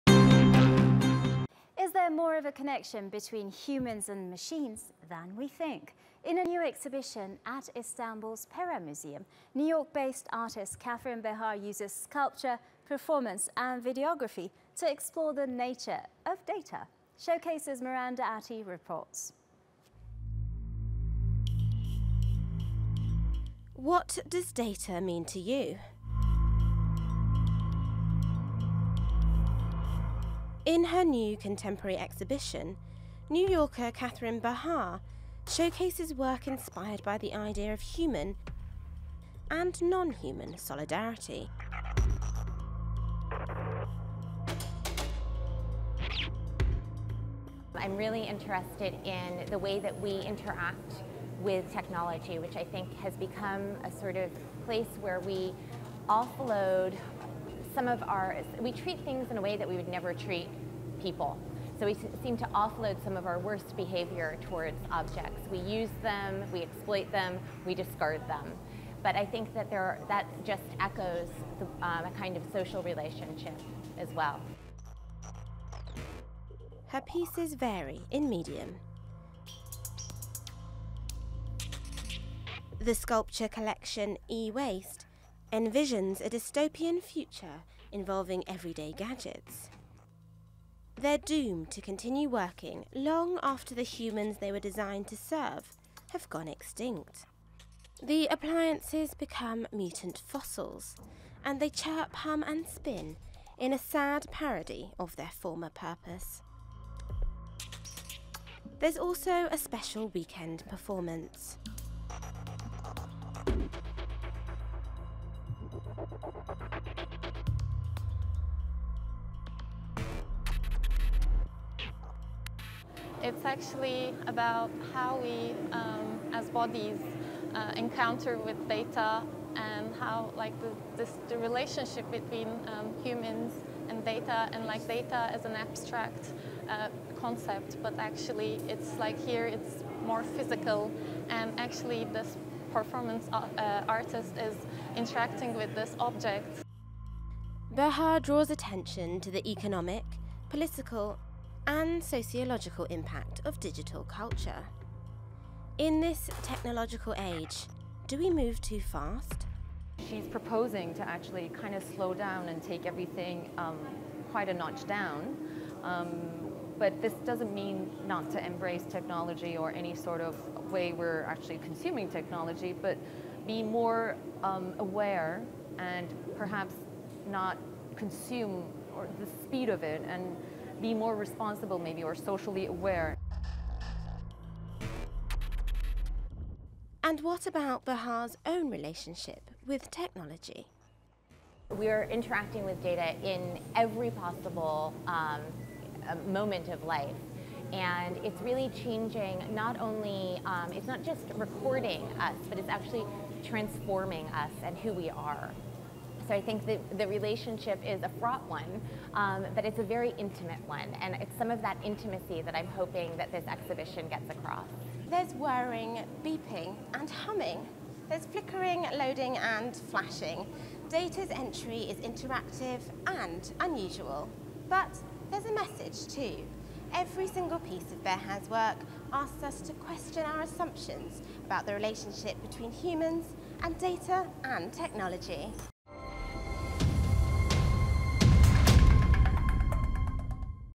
Showcase: Katherine Behar's Exhibition "Data's Entry"
Is there more of a connection between humans and machines than we think? In the exhibition at Istanbul's Pera Museum, New York-based artist Katherine Behar uses sculpture, performance and videography to explore the nature of data.